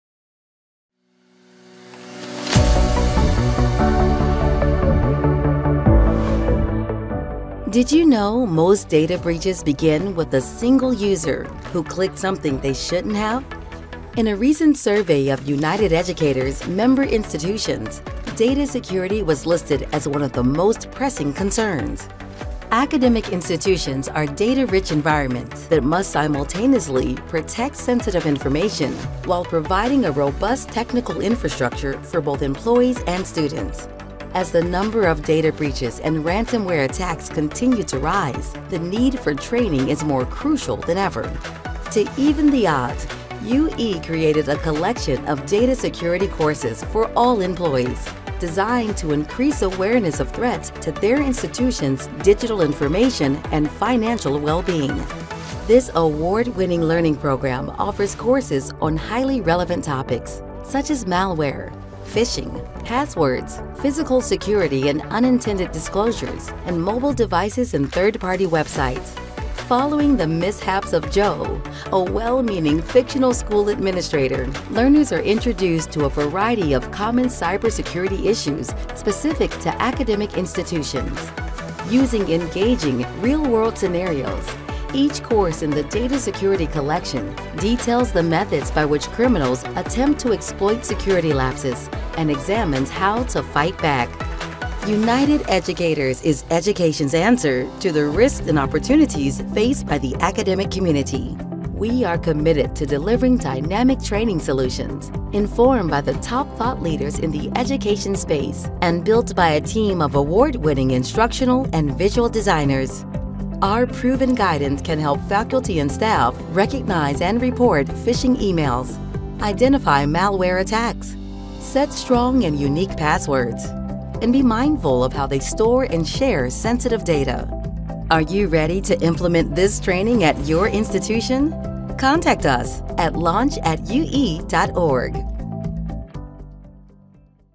Data Security Course Collection Trailer
Equip the faculty and staff at your institution with a high-level overview of data security threats and exposures. This award-winning collection includes online courses and supplemental resources that contain practical steps for institutional stakeholders to avoid data breaches. Gold Stevie® Award Winner.